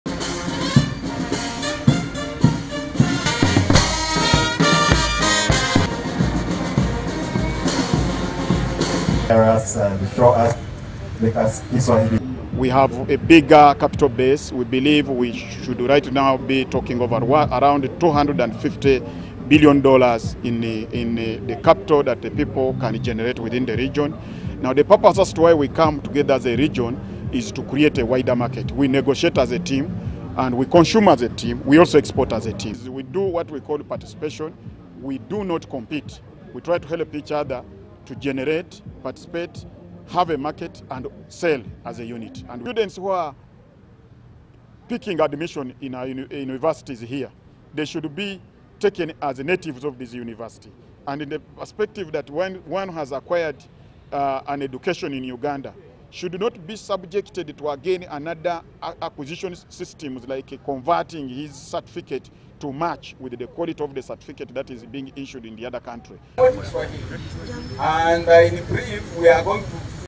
EAC 20 years after Moi, Museveni & Mkapa met
The Ministry of East African Community affairs has today marched from City square to Sheraton Kampala where they are commemorating 20 years of the integration.

It 20 years since presidents Daniel Arap Moi of Kenya, Yoweri Museveni of Uganda and Benjamin Mkapa of Tanzania met and agreed to revive the East African community of Nations.

Julius Maganda Wandera the state minister for east African affairs says that despite a number of challenges that they continue to face, the community has registered a number of achievements